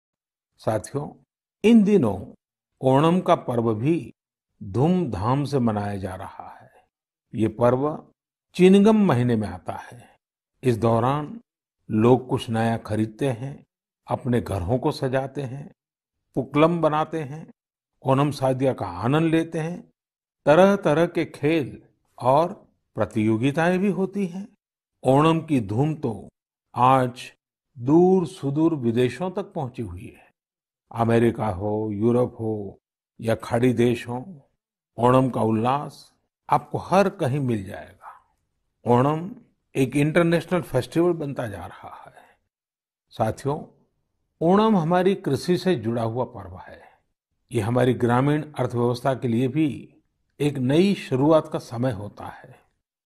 Greetings on Onam. This is a unique festival, which celebrates harmony. It is also an occasion to ex
Greetings on Onam. This is a unique festival, which celebrates harmony. It is also an occasion to express gratitude to our hardworking farmers. May everyone be blessed with joy and best health.